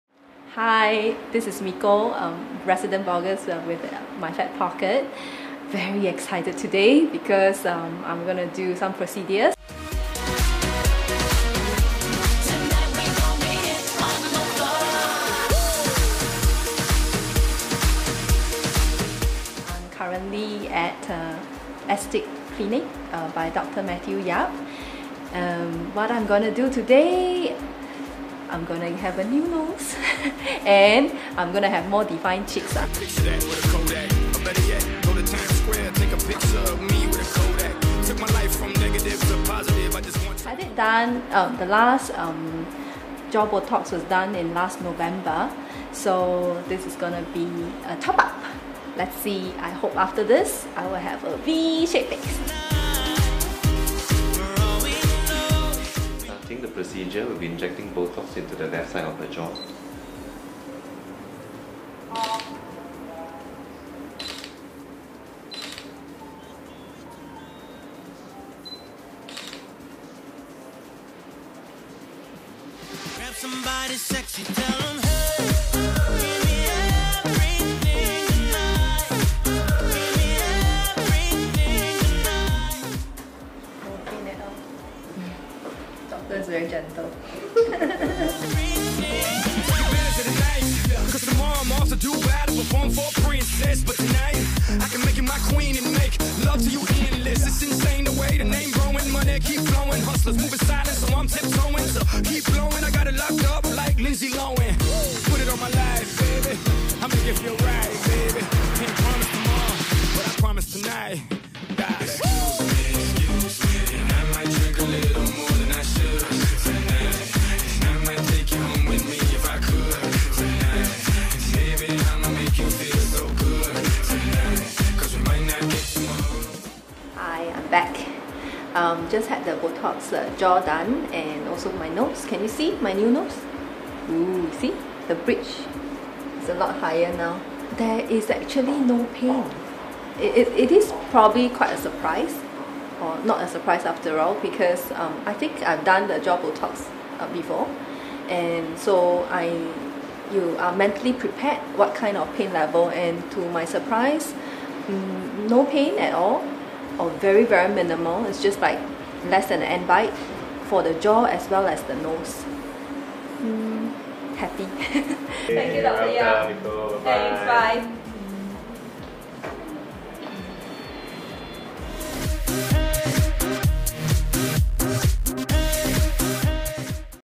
Jaw Botox & Nose Filler with Blogger Mikko
Blogger Mikko did a live session of jaw botox and nose filler with Dr Matthew Yap of Astique Clinic.

ASTIQUE CLINIC
391 Orchard Road #05-35 Ngee Ann City Podium Tower Singapore 238872.